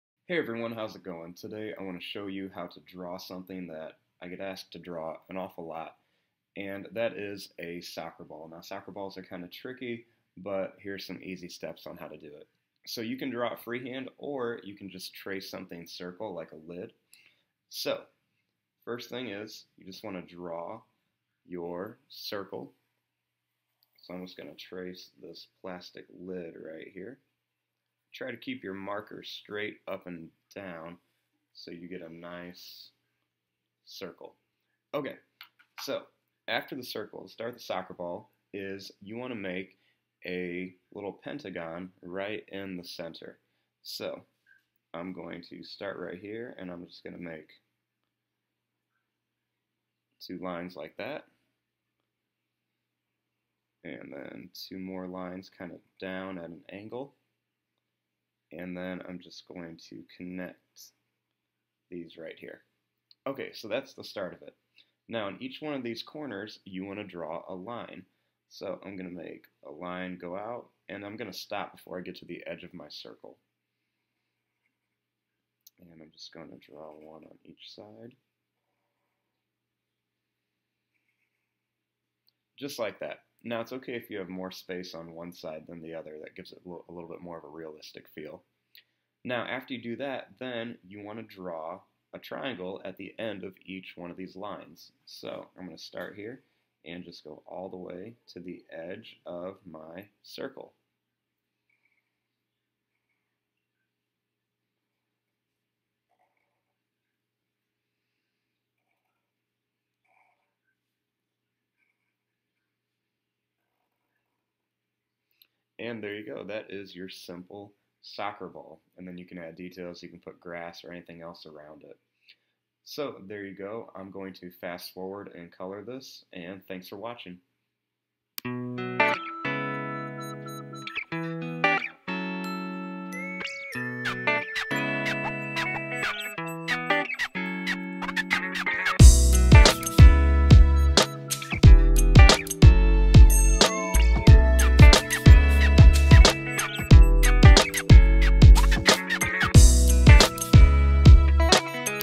How to Draw a Soccer Ball
Easy step by step video on how to draw a soccer ball!